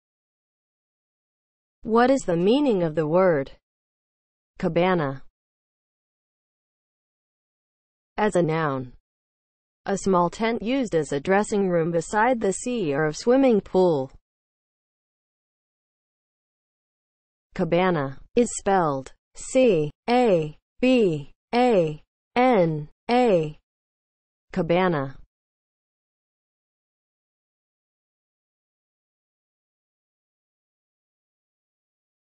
What is the meaning of the word CABANA?
Definition and spelling of the word CABANA.

(00:00) Intro
(00:07) As a noun
(00:16) Spelling

As a noun (00:07):
• a small tent used as a dressing room beside the sea or a swimming pool